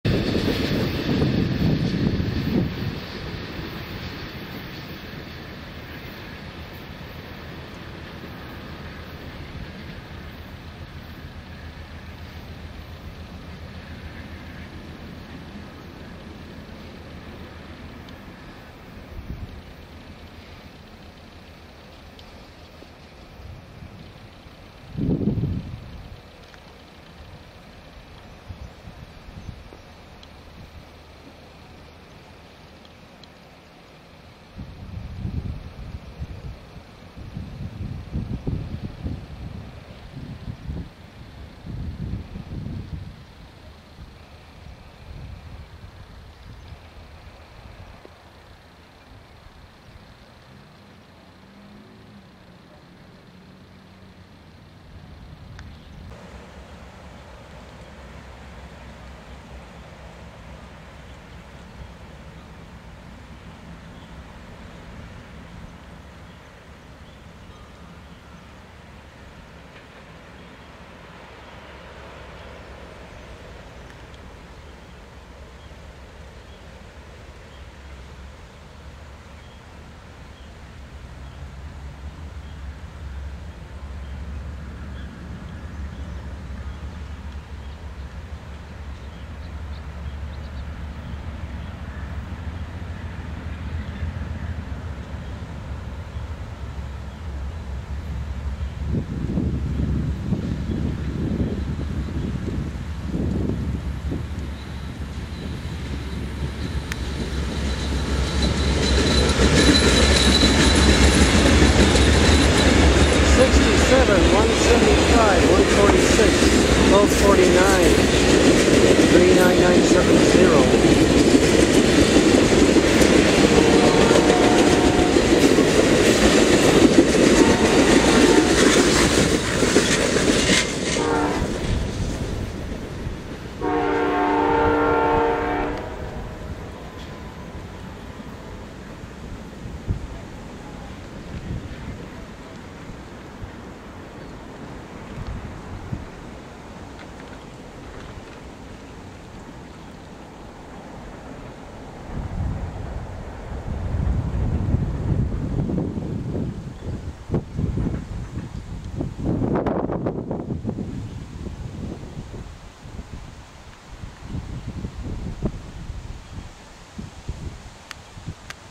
Amtrak #11 and #14 of Tue 1 Nov 2011 [XQ]
Coast Starlights through south- central Atascadero CA: (1) Going- away grab shot of sb. #11 at 2:12 PM (I got caught napping); (2) Nb. #14 at 4:33 PM.